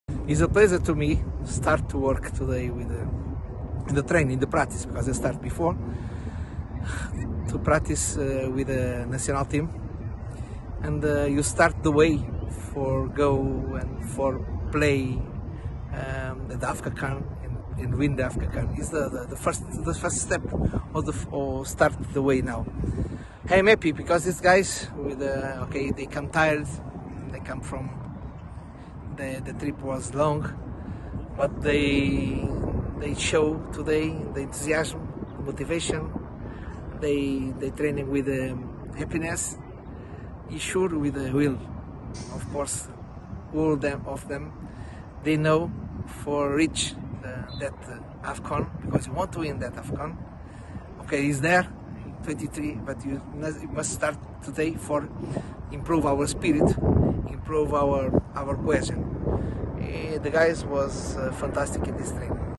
Jose Peseiro: New Super Eagles coach talks tough ahead of 2023 AFCON
The new Portuguese manager  has hit the ground running with the Super Eagles